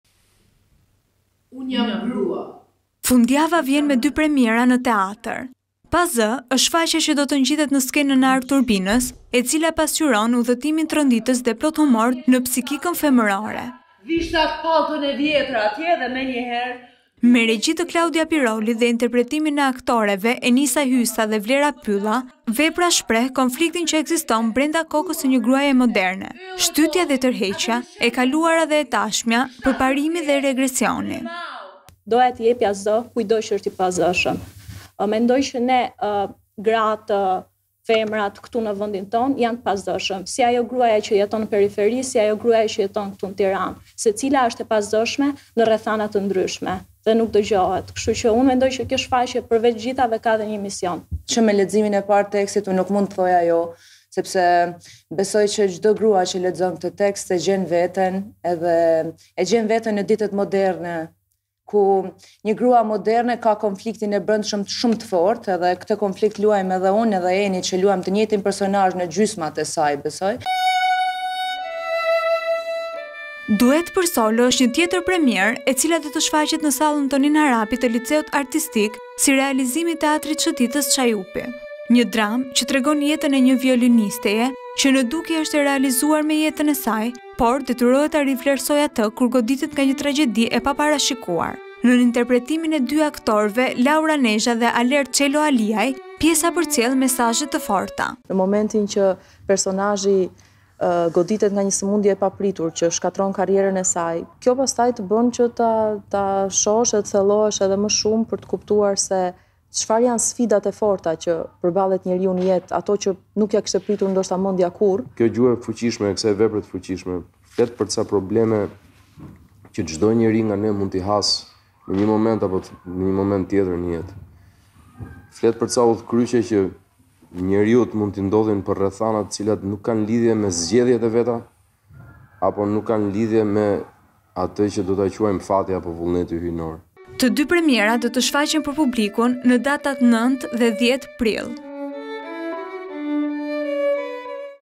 Dy premiera kete fundjave - Vizion Plus
Fundjava vjen me dy premiera në teatrët. “PA ZË” është shfaqja që do të ngjitet në skenën e Arturbinës, e cila pasqyron  udhëtimin tronditës dhe plot humor në psikikën femërore.
Me regji të Klaudia Pirolit dhe interpretimin e aktorëve Enisa Hysa dhe Vlera Pylla, vepra shpreh konfliktin që ekziston brenda kokës së një gruaje moderne: shtytja dhe tërheqja, e kaluara dhe e tashmja, përparimi dhe regresioni.
KLAUDIA PIROLI
REGJISORE

ENISA HYSA
AKTORE
 “Duet për solo”, është premiera tjetër, e cila do të shfaqet në sallën Tonin Harapi të liceut artistik si realizim i Teatri Shëtitës “Çajupi”.
Një dramë që tregon jetën e një violinisteje, që në dukje është e realizuar me jetën e saj,  detyrohet ta rivlerësojë atë kur goditet nga një tragjedi e paparashikuar.Nën interpretimin e dy aktorëve Laura Nezha dhe Alert Çeloaliaj pjesa përcjell mesazhe të forta.
LAURA NEZHA
AKTORE
ALERT ÇELOALIAJTë dy premierat do të shfaqen për publikun në datat  9 dhe 10 prill .